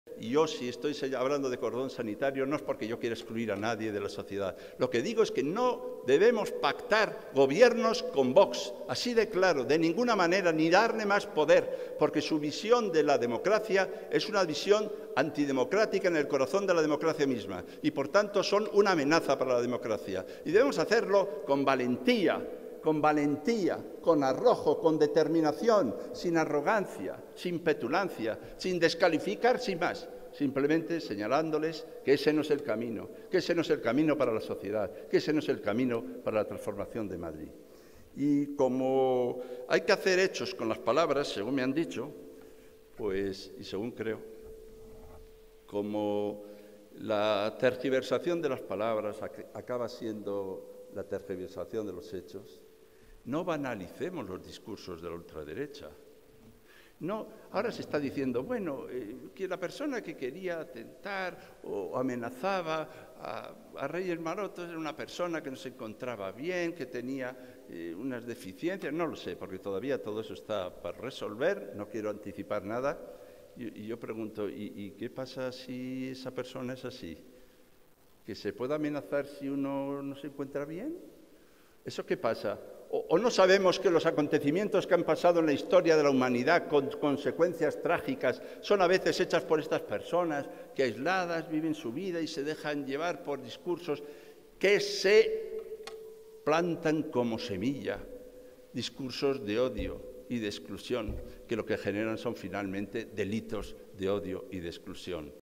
Gabilondo pide no banalizar el discurso de la ultraderecha
El candidato del PSOE a la Presidencia de la Comunidad de Madrid, Ángel Gabilondo, ha pedido este lunes no "banalizar" el discurso de la ultraderecha y no justificar amenazas por el hecho de que una persona "no se encuentre bien" cuando se dejan llevar "por discursos de odio".
(Fuente: PSOE)